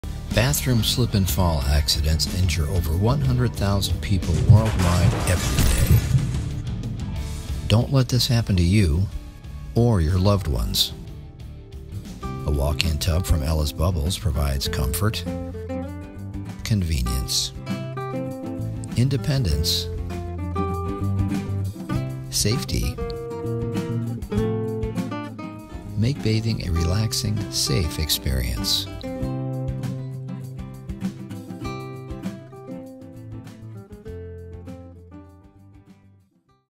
Ella's Bubbles: Walk In Bathtubs with Door/Seat for Elderly - Reduce Slip/Fall while Bathing- Video
Highlight:
0:00 Ella's Bubbles: Walk In Bathtubs with Door/Seat for Elderly - Reduce Slip/Fall while Bathing- Video
0:04 Over 100,000 People Get Hurt In A Bathtub Accident Every Day
0:12 Walk-In BathTubs From Ella's Bubbles Provide Comfort, Convenience, Independence, And Safety
0:30 Subscribe To Ella's Bubbles

Ella’s Bubbles walk-in bathtubs provide a safe bathing experience. All Ella brand bathtubs are equipped with safety features, such as a low step-in threshold for entry and exit, deck and wall mount safety grab bars, textured floor, and built-in contoured molded seat. Additionally, some models have a distinct door to allow an easier transition from wheelchair to tub. Overall, Ella’s walk-in bathtubs are the perfect choice for bathers who may need accommodations while bathing.